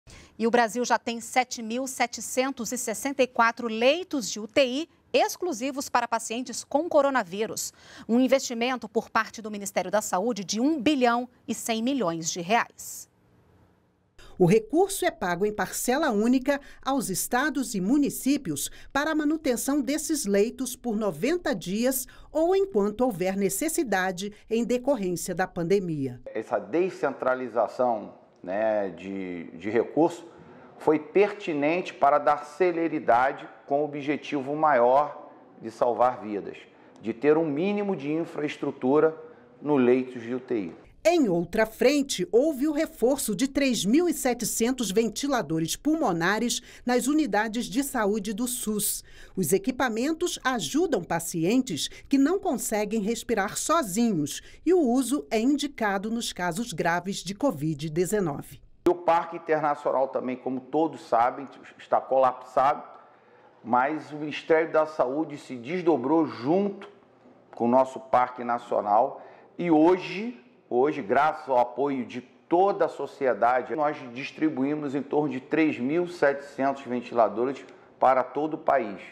Mais de R$ 1,1 bi já foram investidos em leitos de UTI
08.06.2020 - O Brasil já tem 7.764 leitos de UTI exclusivos para pacientes com coronavírus. Um investimento por parte do Ministério da Saúde de 1 bilhão e 100 milhões de reais.